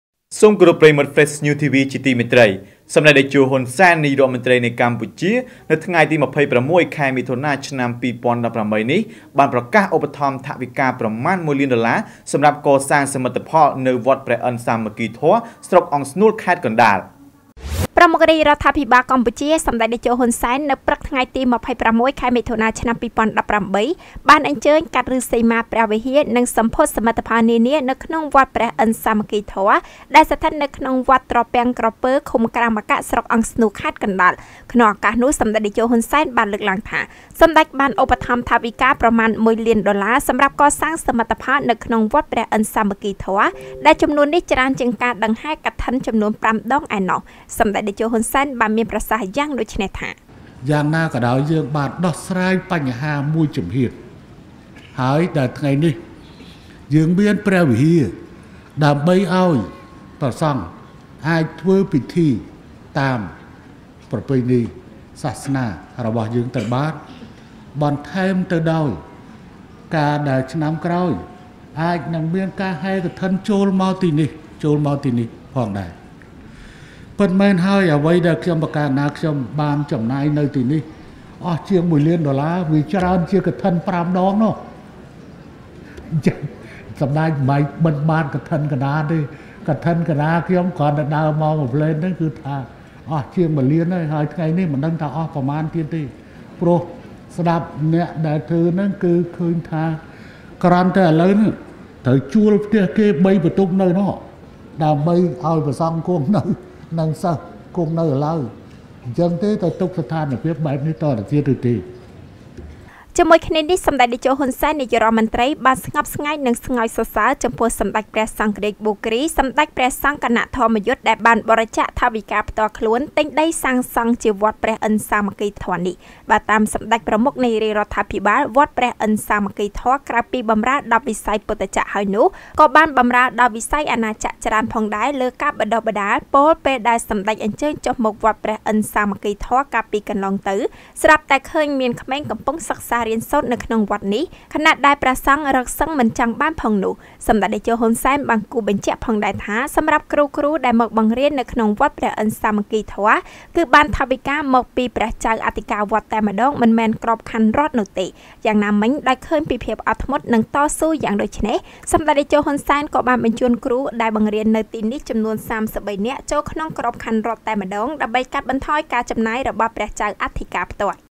សម្ដេចតេជោ ហ៊ុន សែន ឧបត្ថម្ភថវិកាប្រមាណ១លានដុល្លារ សម្រាប់កសាង...
សម្ដេចតេជោ ហ៊ុន សែន ឧបត្ថម្ភថវិកាប្រមាណ១លានដុល្លារ សម្រាប់កសាងសមិទ្ធផលនៅវត្តព្រះឥន្ទសាមគ្គីធម៌ ស្រុកអង្គស្នួល ខេត្តកណ្តាល​ (រាយការណ៍ព័ត៌មានដោយលោក វៃ ជីវ័ន និងកញ្ញា​ យ៉ង់ សក្គុណា​, Fresh News TV)